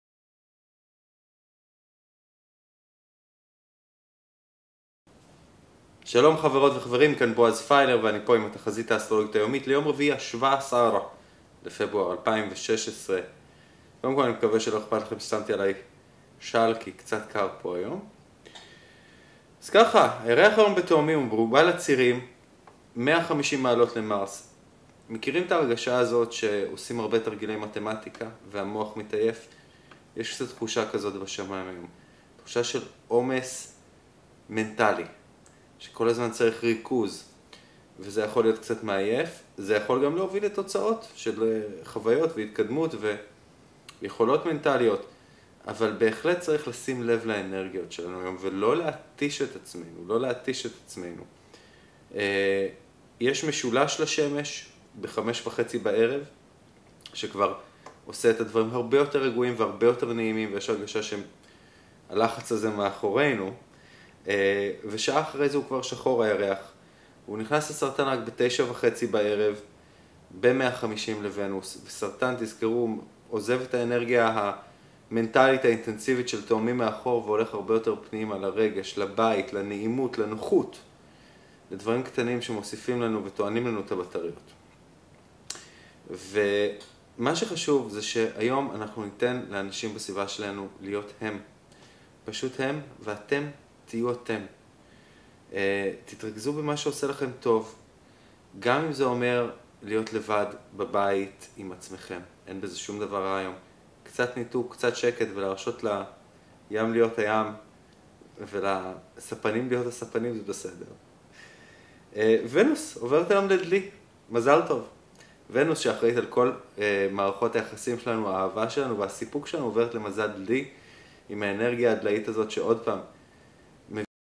התחזית האסטרולוגית היומית עם בועז פיילר 17 פבר 2016
חברות וחברים - איתכן\ם הסליחה. הקובץ קטוע ואם רוצים היום את התחזית המלאה יש לפנות לגרסה האנגלית. תודה וסליחה!